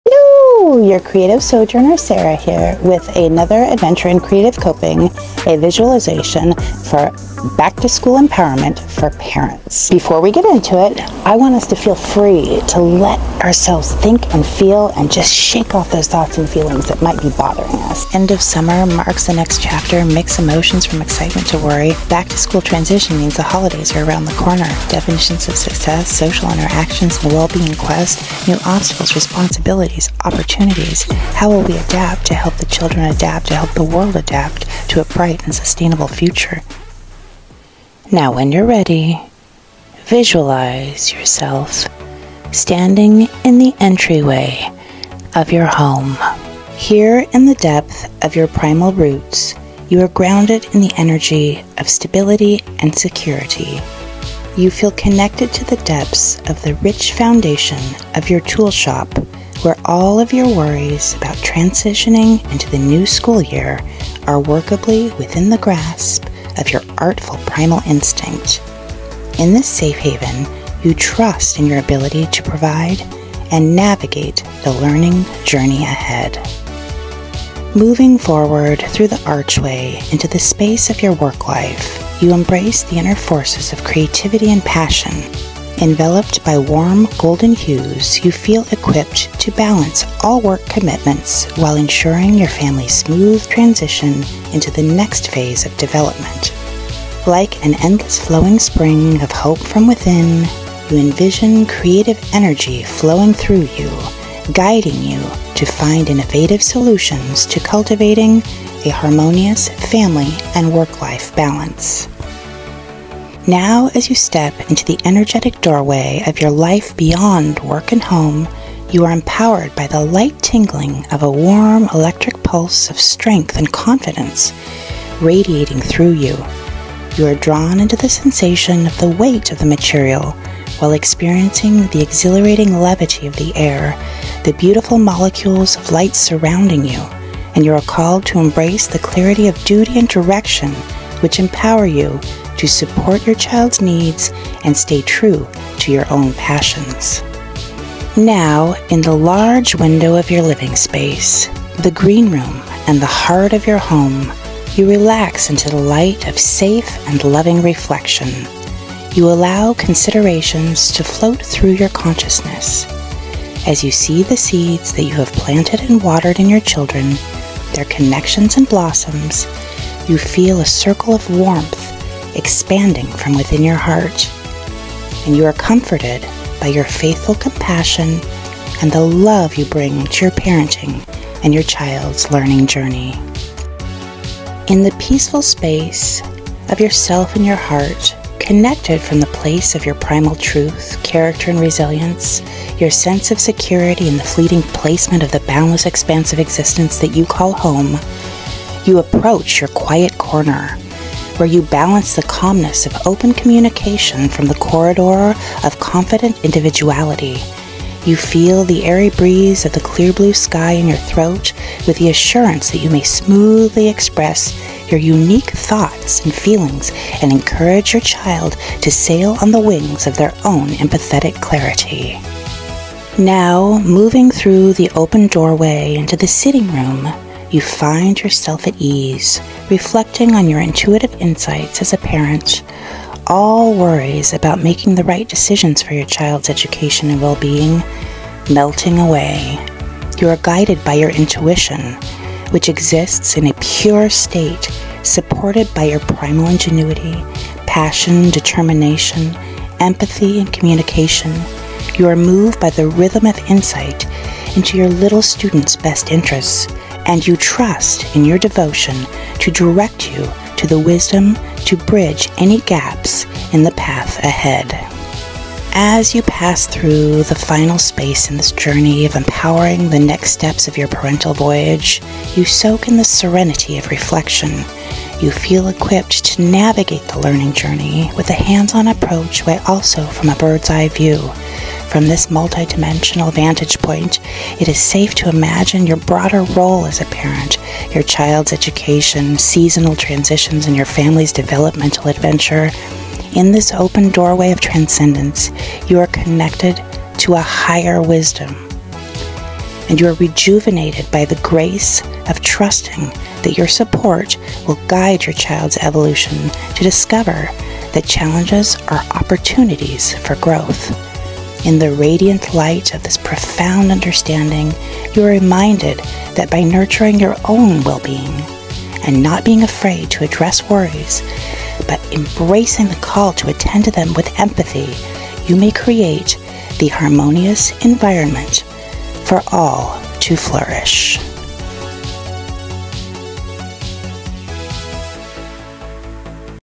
Navigating Back to School Worries with Chakra Balance Visualization: Empowerment for Parents
As the back-to-school season approaches, parents often find themselves on a journey filled with a mix of emotions, ranging from excitement to worry. It's natural to have concerns about your child's transition back to school, academic success, social interactions, and overall well-being. That's why we invite you to embark on a unique and transformative journey through the chakra doorways of your house, work, and life. This journey will provide you with invaluable tools to navigate these worries and obstacles with a balanced and empowered approach. xoxoxo

00:00 Parent empowerment for back to school
00:18 Pre-visualizaton freakout
00:38 Root Chakra for parental empowerment
01:18 Creativity Chakra for parental empowerment
01:53 Confidence Chakra for parental empowerment
02:27 Heart Chakra for parental empowerment
03:04 Voice Chakra for parental empowerment
03:42 Third Eye Chakra for parental empowerment
04:25 Crown Chakra for parental empowerment